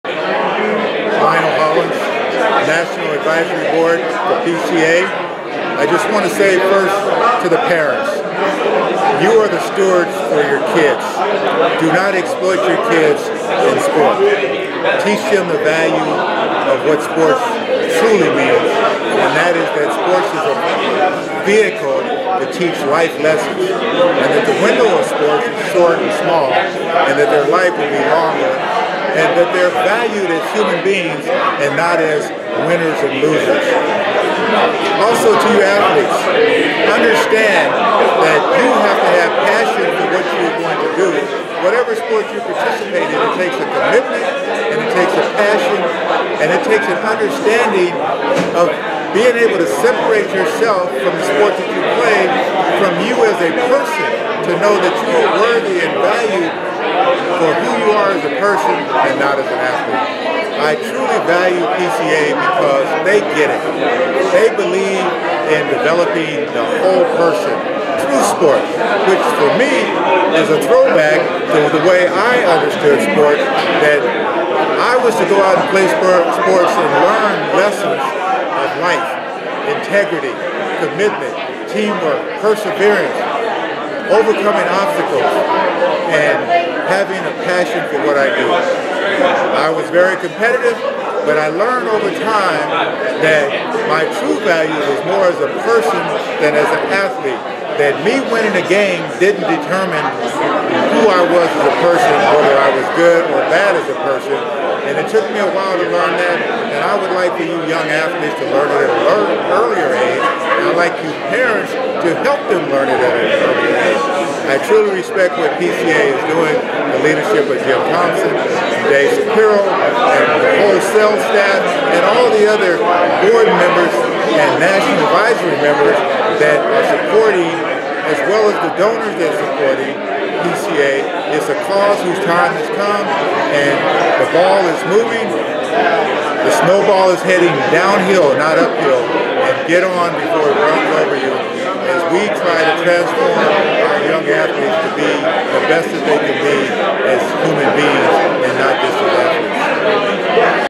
Lionel Hollins Advises Youth Sports Parents
Lionel Hollins, former NBA player and coach and Positive Coaching Alliance National Advisory Board Member, shares advice for sports parents and inspiration for the PCA Movement in this video from PCA's 13th Annual National Youth Sports Awards Dinner and Auction sponsored by Deloitte.